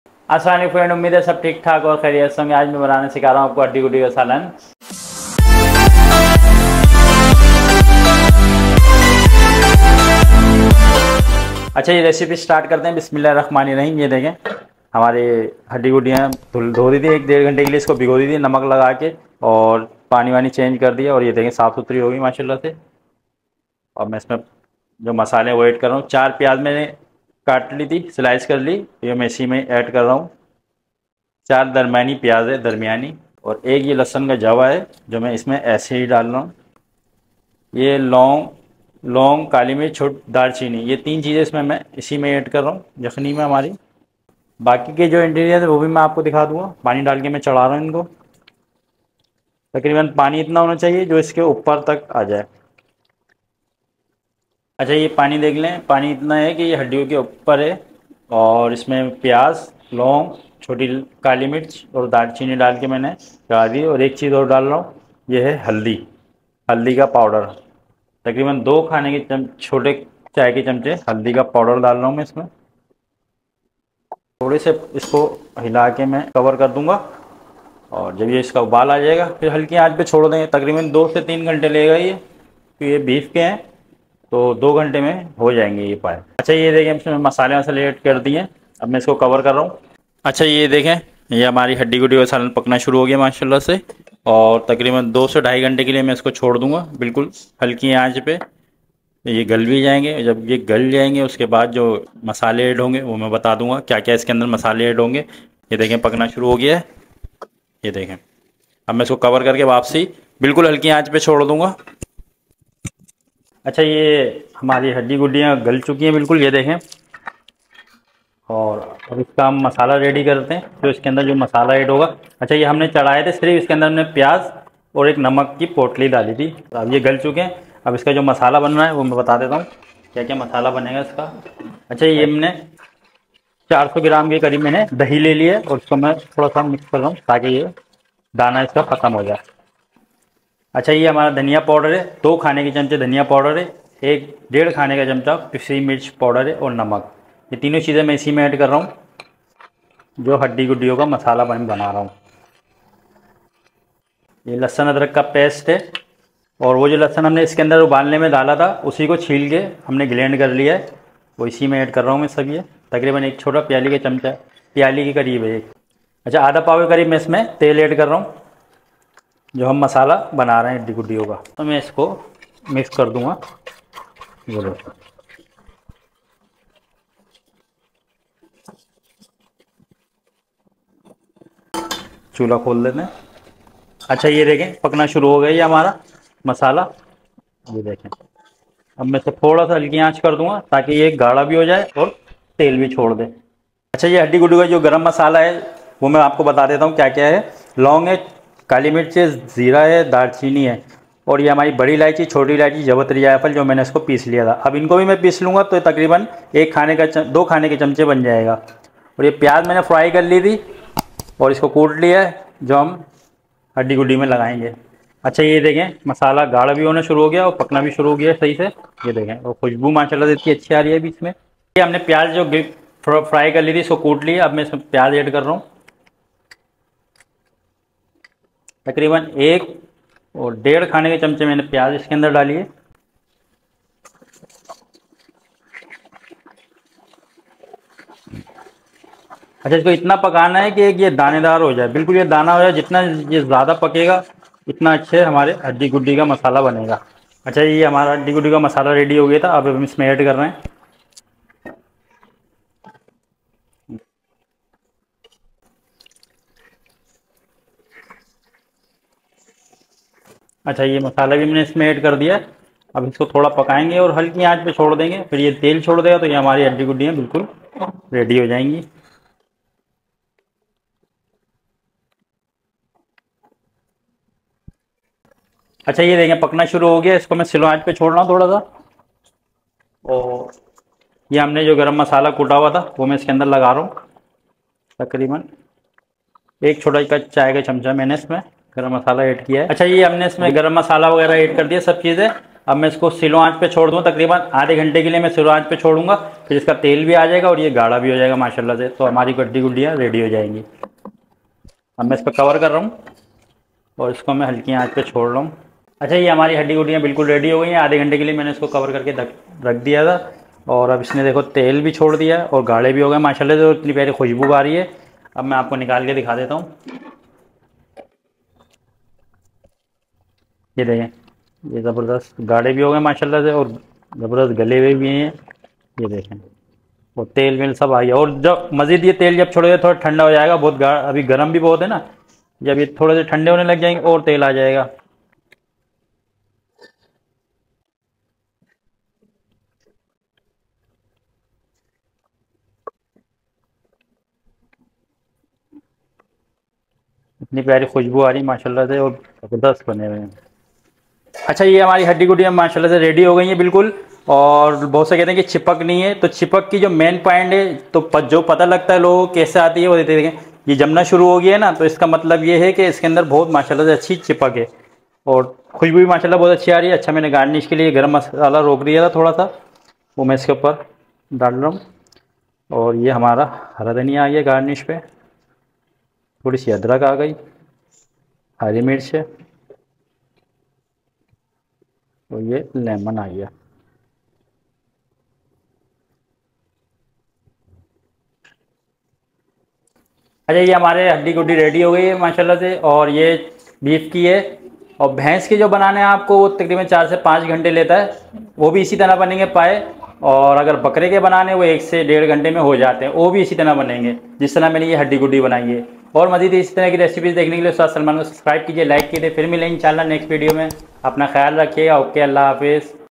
Haddi Guddi Ka Salan Recipe By Ustad Salman | ہڈی گُڈی کا سالن | Taste Like Beef Paya
This is Ustad Salman From Nadeem Pakwan Center.

@ustadsalman937   for healthy food and kids special recipe

please...
hits likes👍🏻
comments👆🏻

Nalli Haddi Guddi Ka Salan Recipe | Nalli